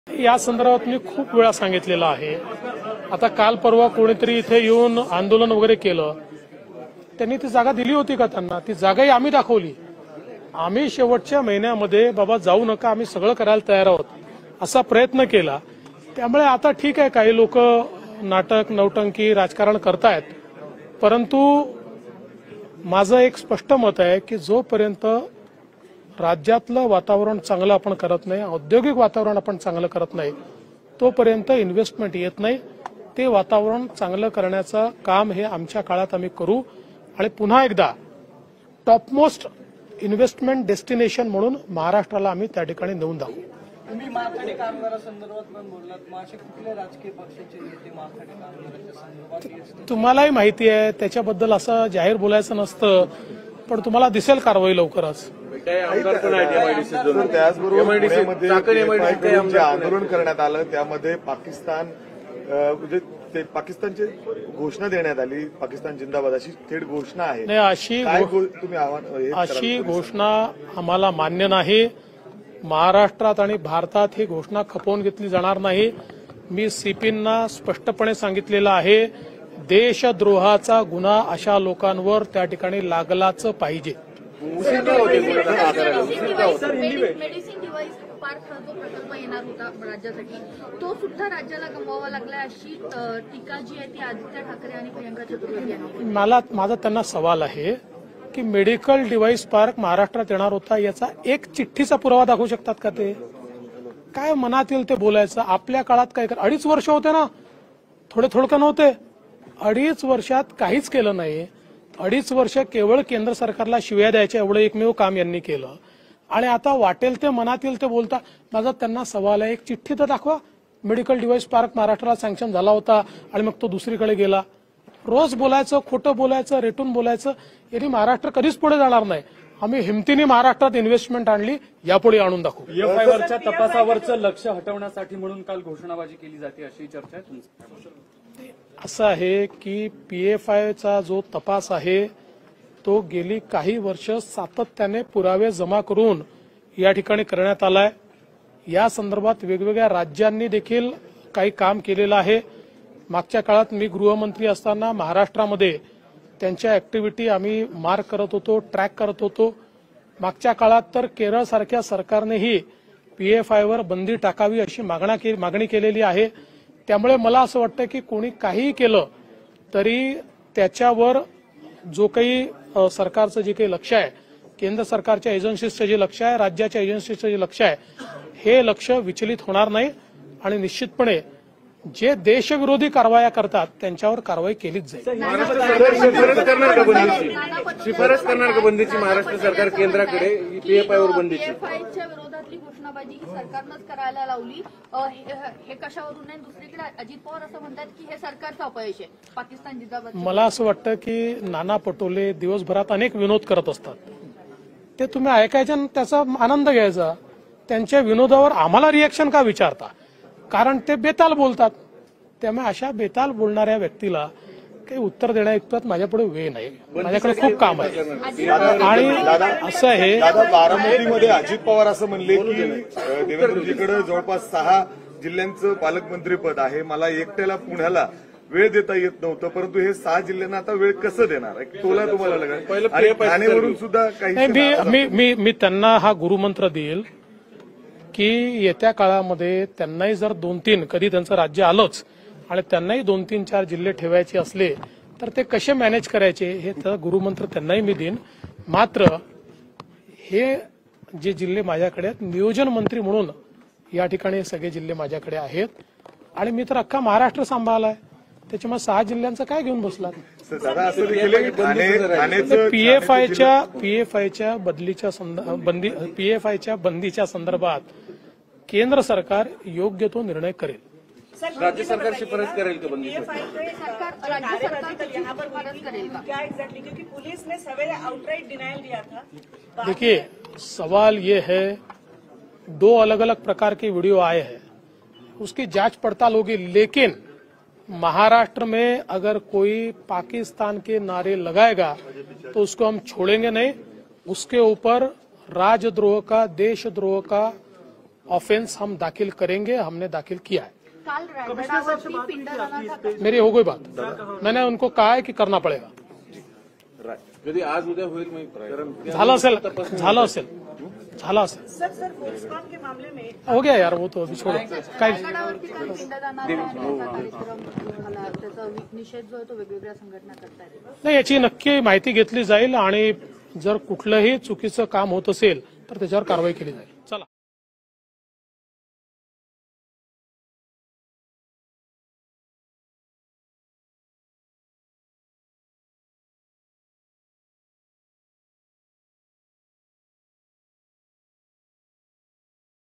Devendra Fadnavis : PFI प्रकरणी गृहमंत्री देवेंद्र फडणवीसांचे आदेश | LetsUpp Marathi
PFIच्या आंदोलनात 'पाकिस्तान जिंदाबाद'च्या घोषणा देणाऱ्यांवर देशद्रोहाचा गुन्हा दाखल करण्याचे आदेश गृहमंत्री देवेंद्र फडणवीस यांनी दिले आहेत.